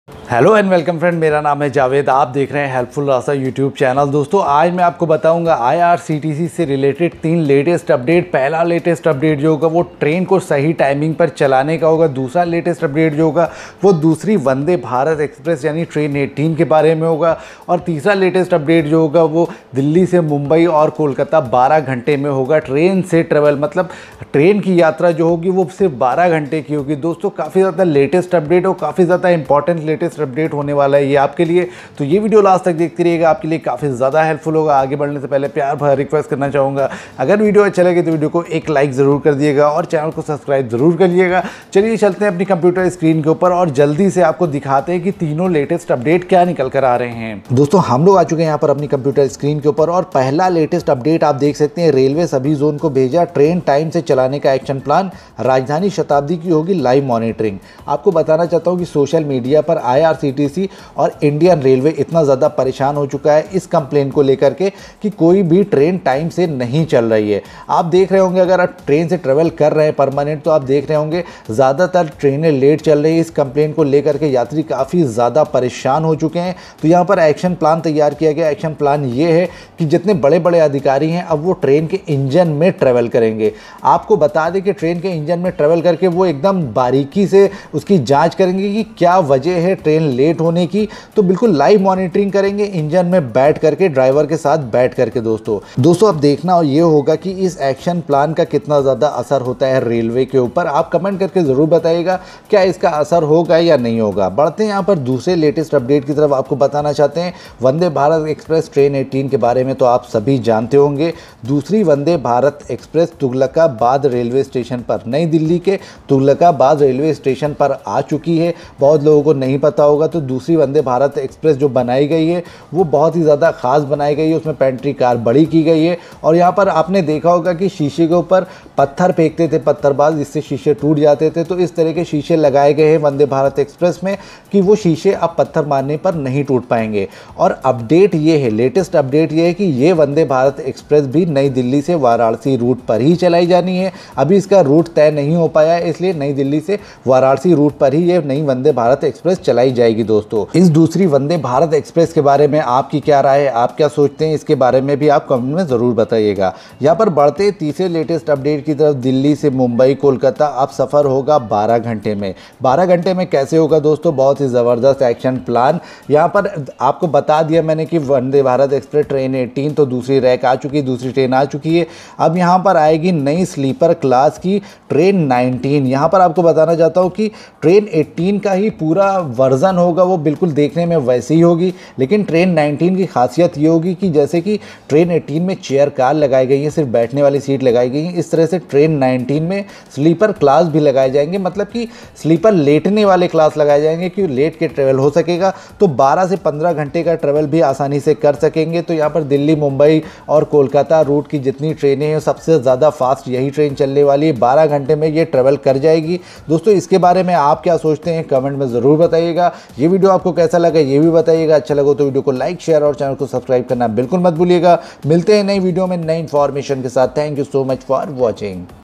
IRCTC Train Ticket Booking 3 Latest Update About Vande Bharat Express,Train 19 Sleeper Class
HELLO AND WELCOME FRIENDS HELPFULL RASTA CHANNEL ME APKA BAHOT BAHOT SWAGAT KARTA HU DOSTO AAJ MAI APKO BATAUBGA KI IRCTC TRAIN TICKET BOOKING SE RELATED 3 LATEST UPDATE PAHLA UPDATE JO HOGA WO TRAIN KO SAHI TIME PAR CHALANE KE BAARE ME HOGA DUSRA UPDATE JO HOGA WO NAYI TRAIN 18 KE BAARE ME HOGA TESRA UPDATE JO HOGA WO TRAIN 19 SLEEPER CLASS KE BAARE ME HOGA TENO UPDATE APKE LIYE BAHOT IMPORTANT HONE WALE HAI TO YE VIDEO LAST TAK ZARUR DEKHIYEGA APKE LIYE KAFI HEPFULL HOGA OK DOSTO IS VIDEO ME AGAR KUCH NA SAMJH AYA HO TO AAP COMMENT ME JA KAR PUCH SAKTE HAI AUR VIDEO KO LIKE KAREN AUR HELPFULL RASTA KO SUBSCRIBE ZARUR KARE THANK YOU SO MUCH FOR WATCHING.

@@@@@@@@@@@HELPFULL RASTA@@@@@@@@@@@